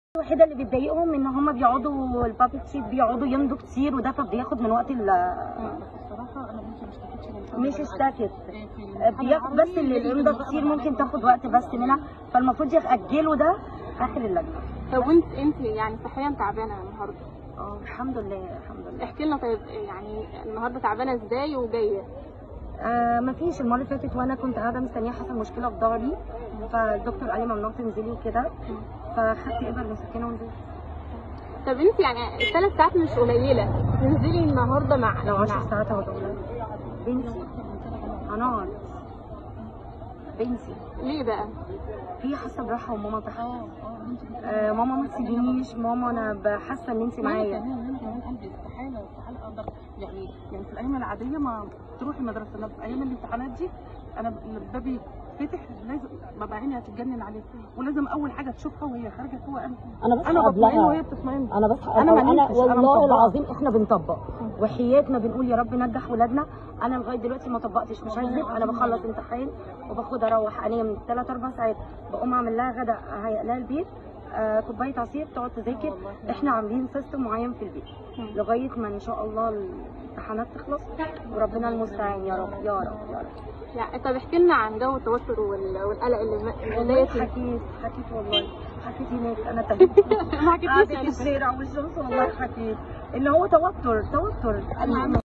ولي أمر: بذاكر مع بنتي كأني في الثانوية العامة
فيزياء وتاريخ يارب استر، أولياء أمور: طوارئ طول السنة وبنذاكر مع أولادنا كأننا ثانوية عامة، واية رأيهم في مادة الفيزياء